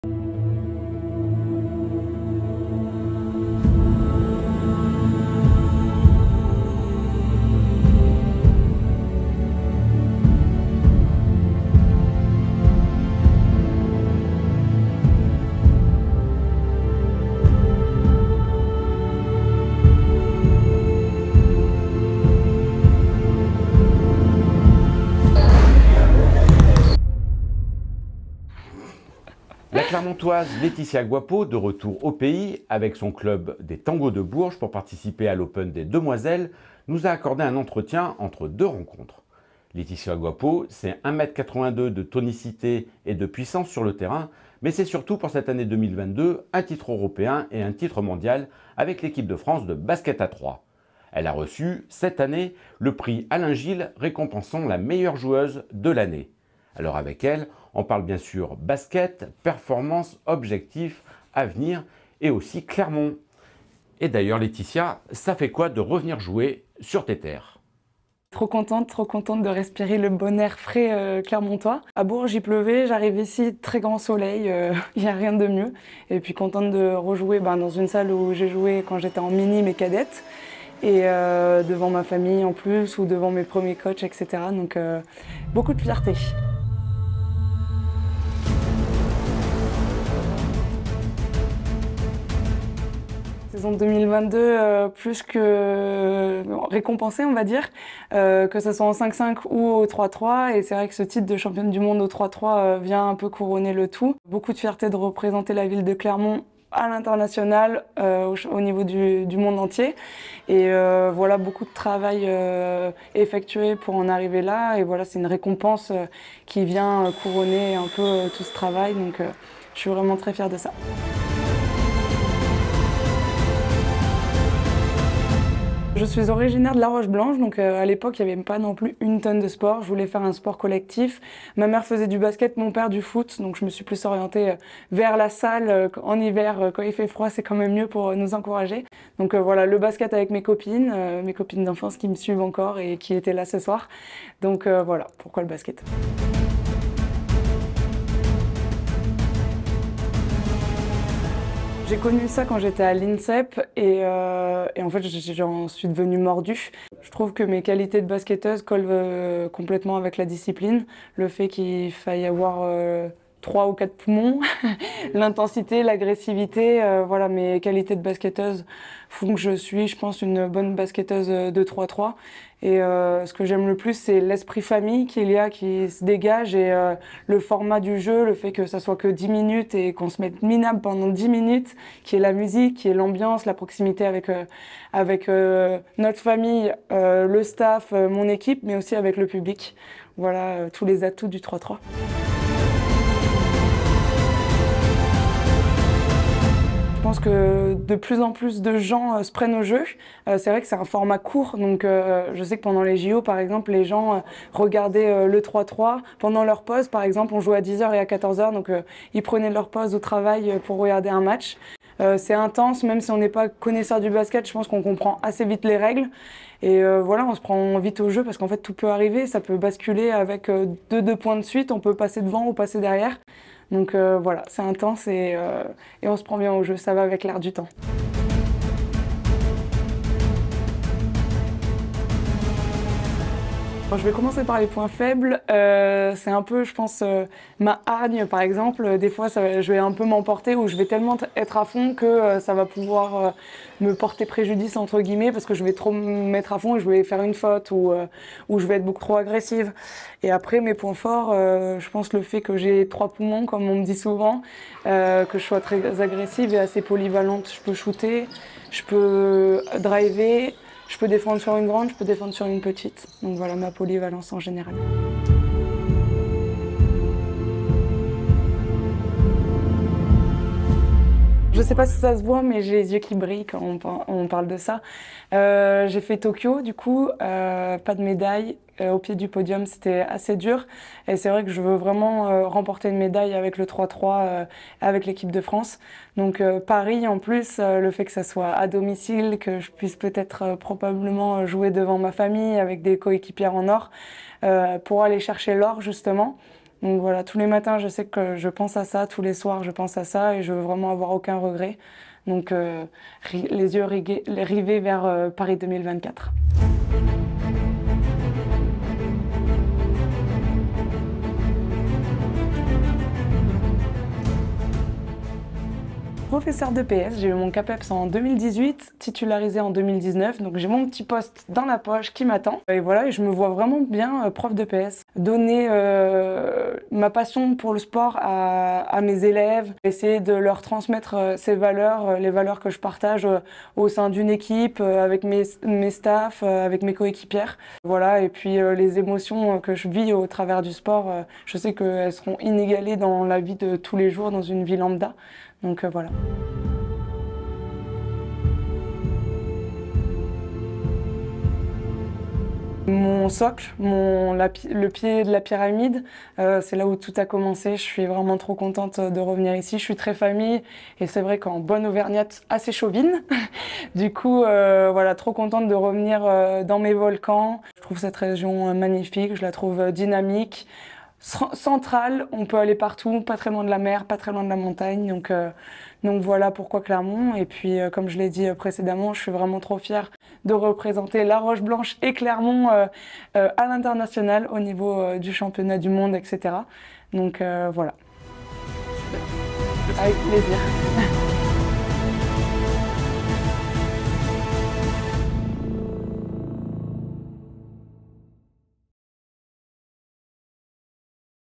Laëticia GUAPO, UNE CHAMPIONNE SUR SES TERRES
Rencontre avec la clermontoise Laëtitia Guapo entre deux matches de l'Open des demoiselles. La championne du monde de basket 3x3 a les yeux qui brillent quand elle parle de son objectif les JO 2024 mais aussi de sa ville Clermont.